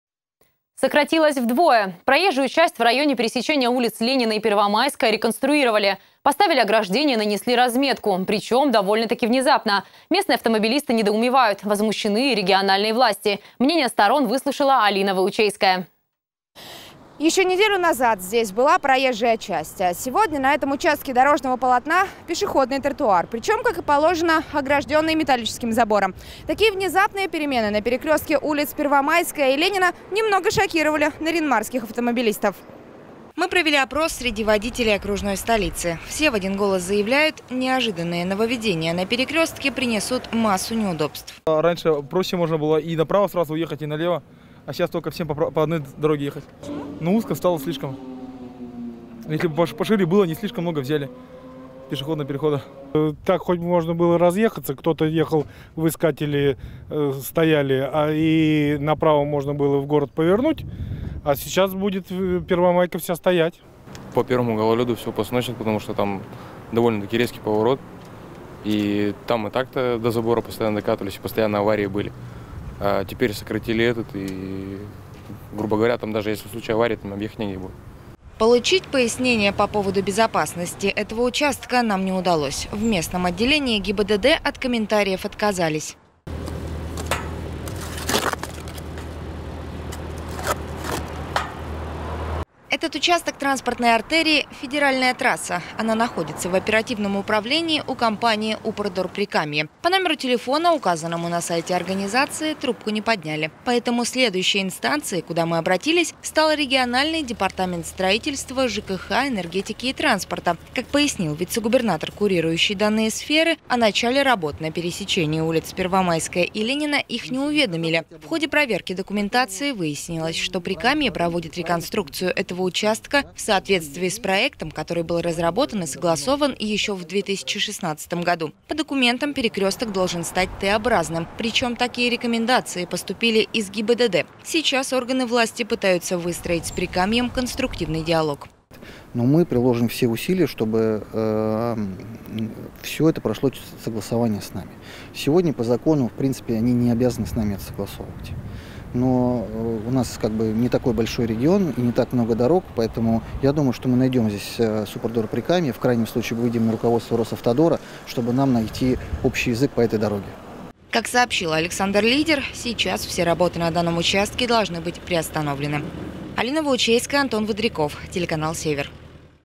ЧТО БУДЕТ С КОЛЬЦЕВОЙ ДОРОГОЙ?
Сократилась вдвое. Проезжую часть в районе пересечения улиц Ленина и Первомайской реконструировали. Поставили ограждения и нанесли разметку... Причем, довольно таки внезапно. Местные автомобилисты недоумевают. Возмущены и региональные власти. Мнения сторон выслушала Алина Выучейская.